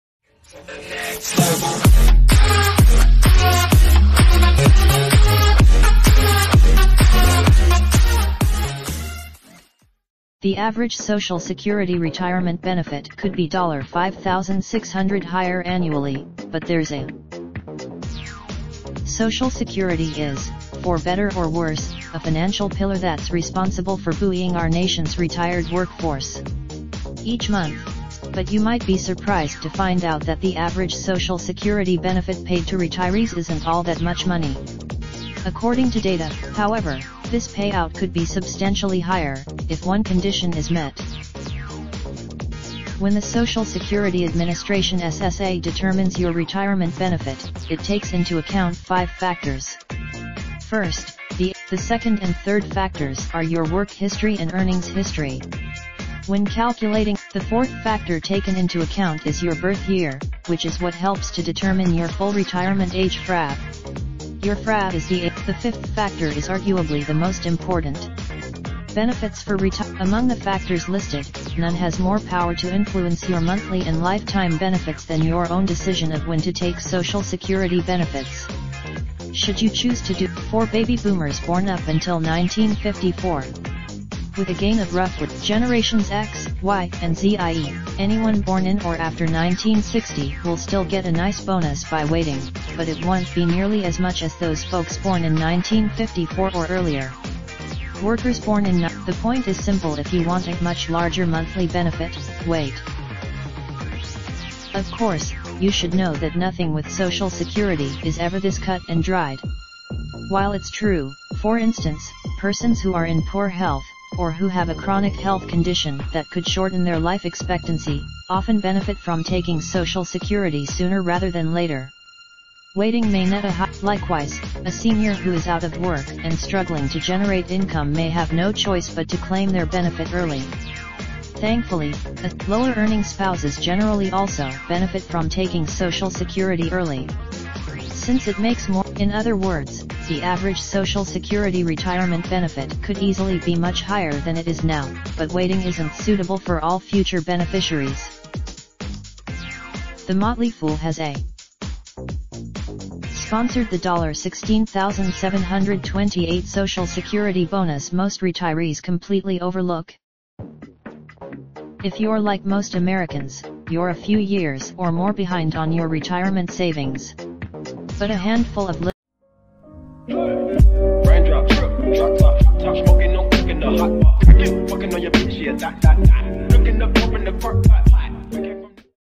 The Average Social Security Retirement Benefit Could Be dollar 5,600 Higher Annually, but Theres a
The Average Social Security Retirement Benefit Could Be  dollar 5,600 Higher Annually, but Theres a Catch